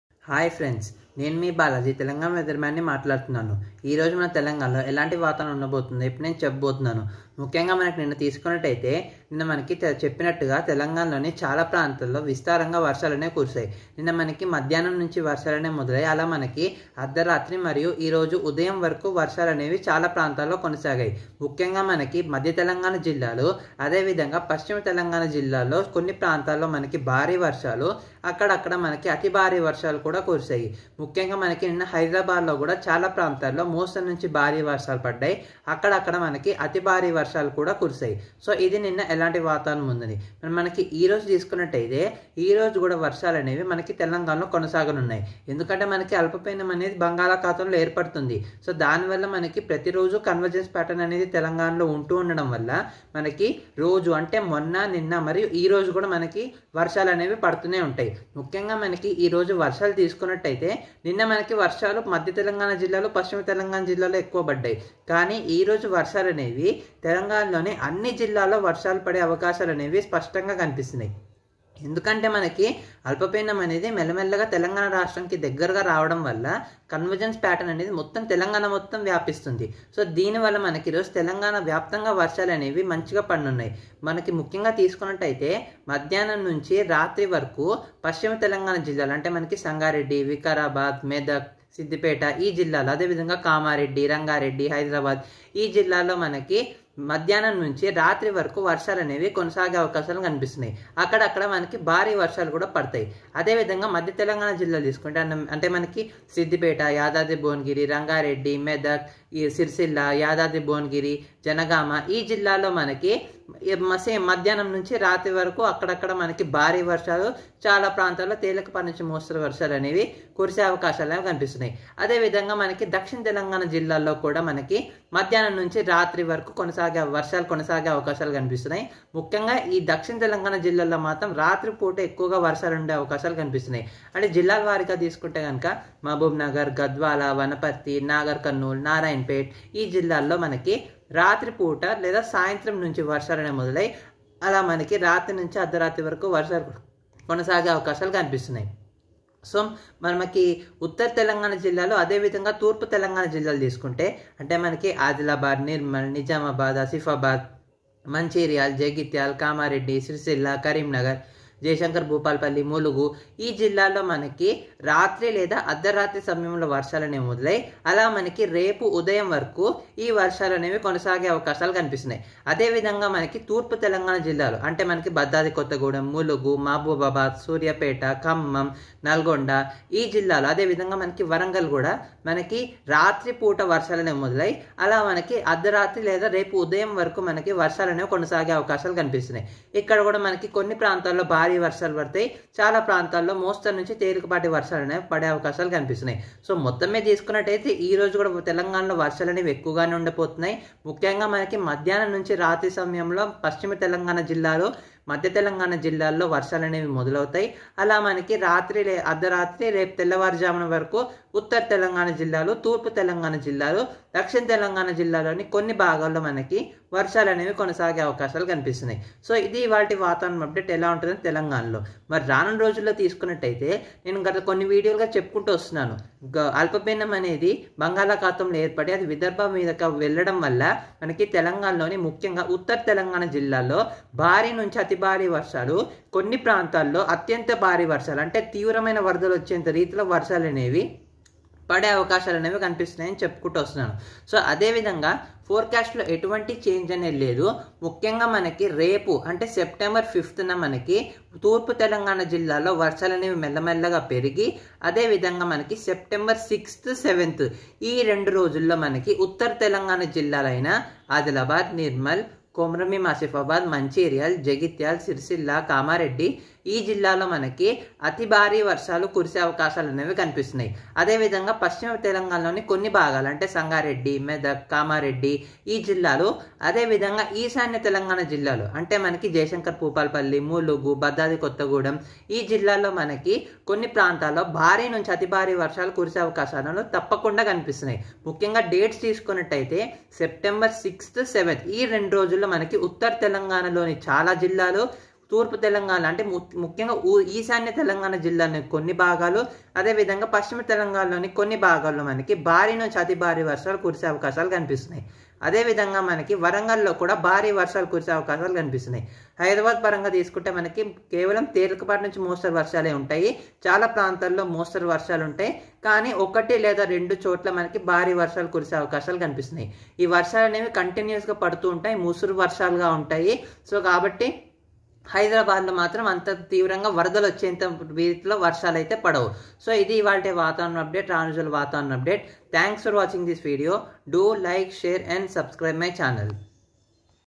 Flooding Rains in North Telangana ⚠️ | September 5 Forecast | Telangana Weatherman
Hi, I am Balaji, Telangana Weatherman here giving you the accurate weather forecasts for Telangana state.